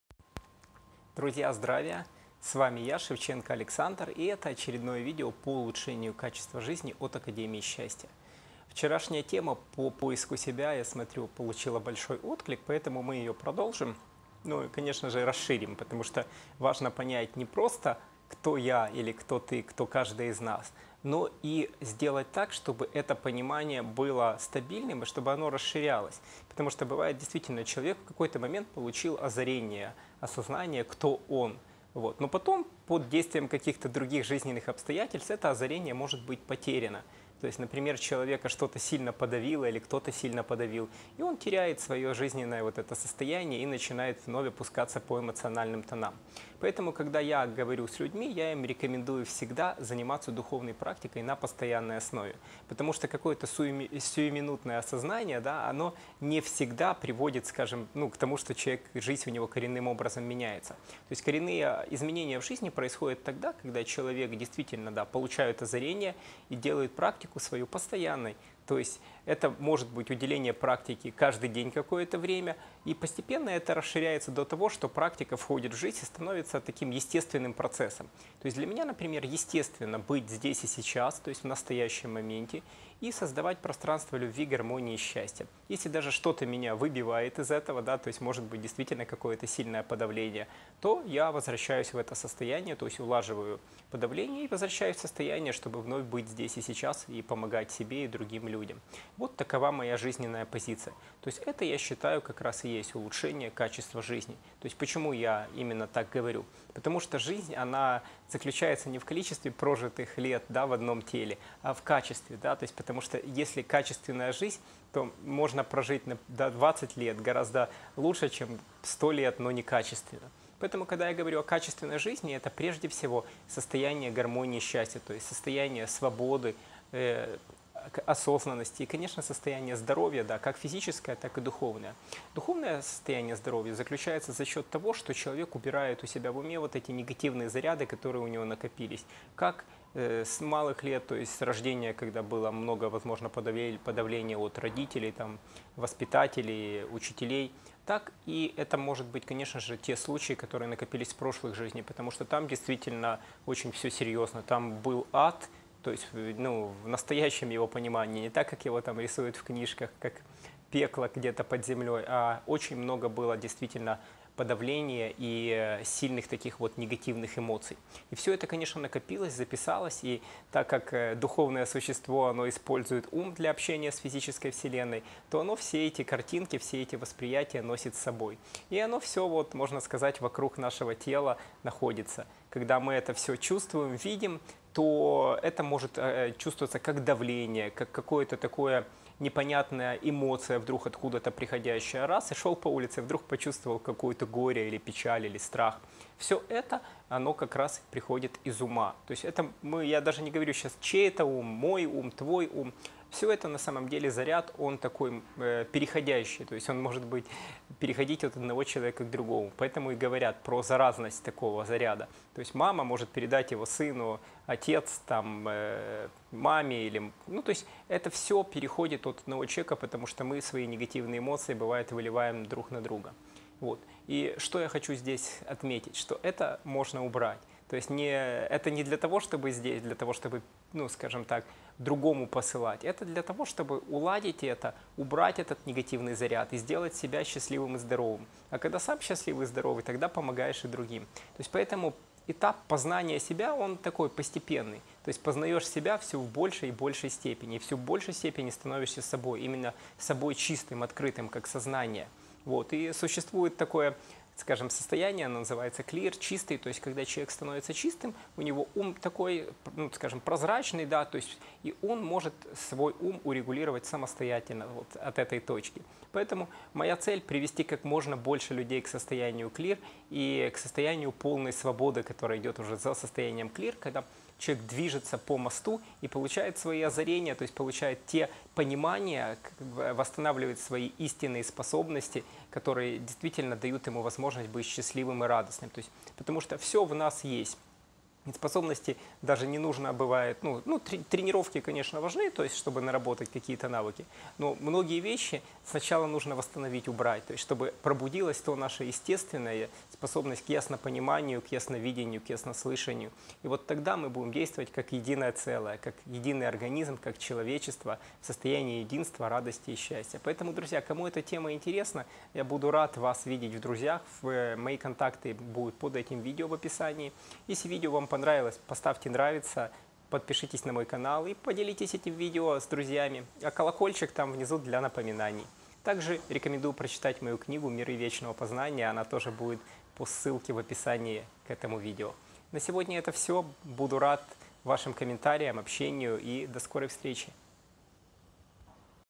Расширенное осознание
Расширенное осознание - это, что каждый хочет достичь.
Вот только, как это сделать? Вопрос такой возникает у многих.
Кто-то занимается занимается духовное практикой, кто-то употребляет вещества. Некоторые вообще не задумываются об этом.
Цель в данном случае оправдывает средства. Важно превратить свою жизнь в практику.
И тогда она станет осознанной и счастливой. Во благо!